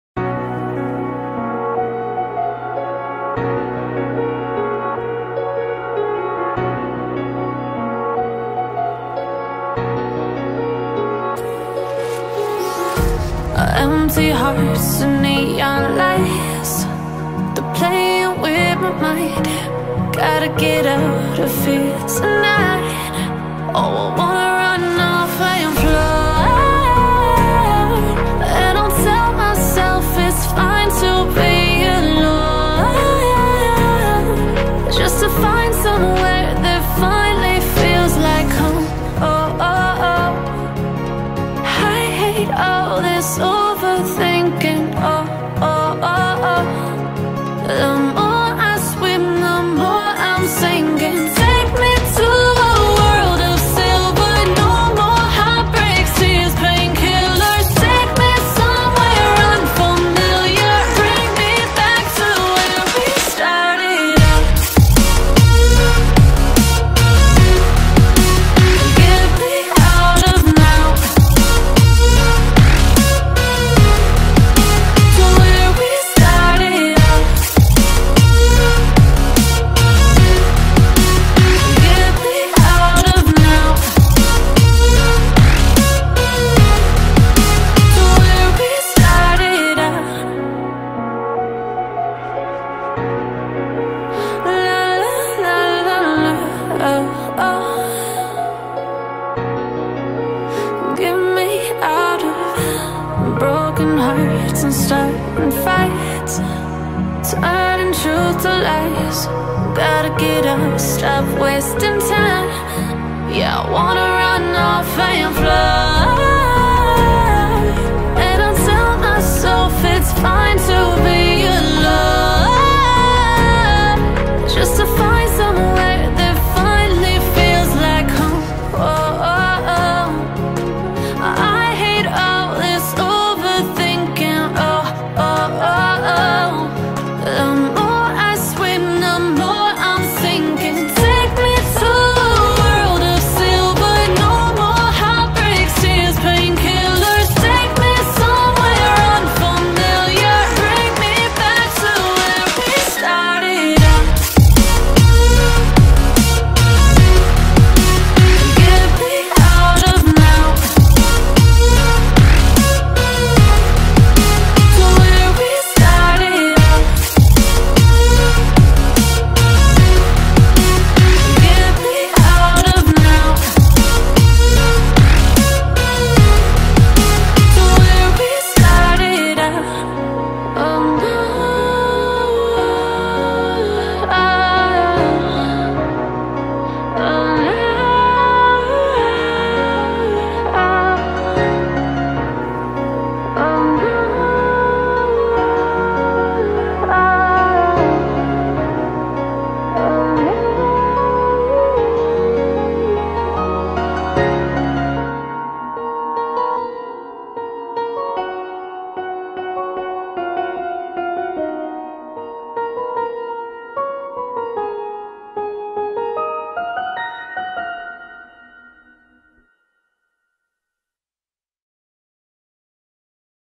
(134,136) Entire S Set For 1 coin repair only,build at the end of video.
Here is the video for 134 and 136 target path,

I managed to do a 136 for 13 minute in base repair and 134 target for 19 min in base repair so that put my flag at 13+19=32 minute repair and that was 1 coin+instant repair for entire set,

134 target was mostly auto,136 i completely driven

i have shown build at the end of video,